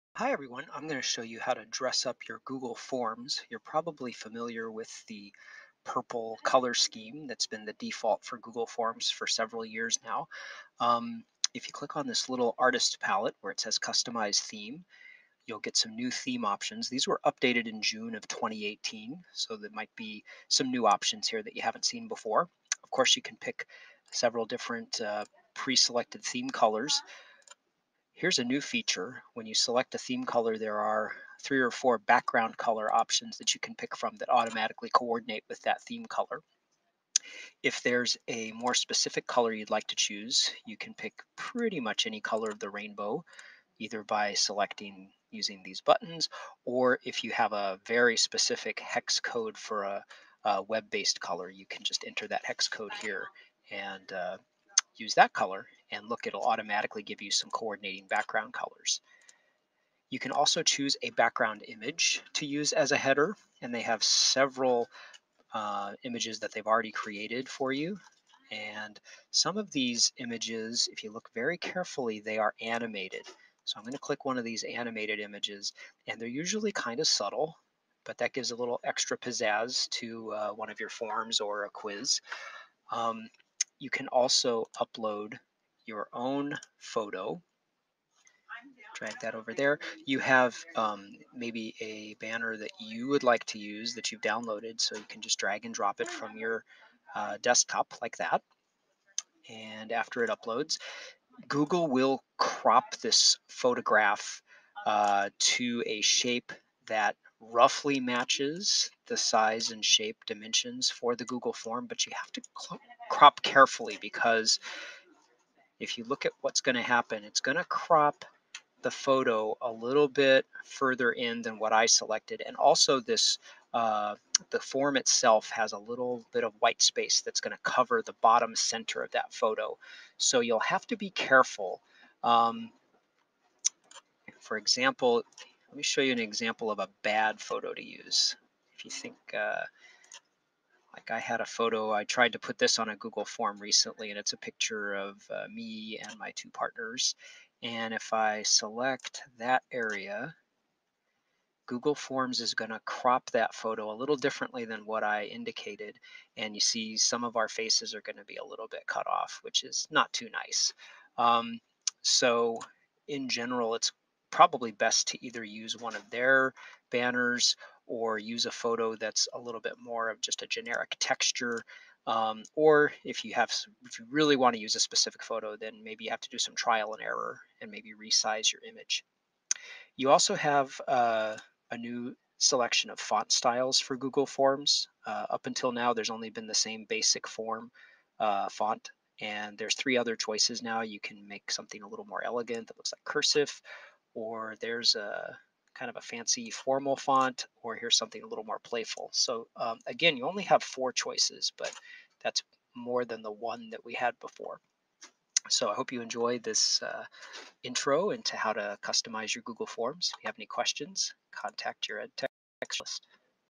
Dress Up Your Google Forms
Summary of the Google Forms Customize Theme options, which were updated in June 2018.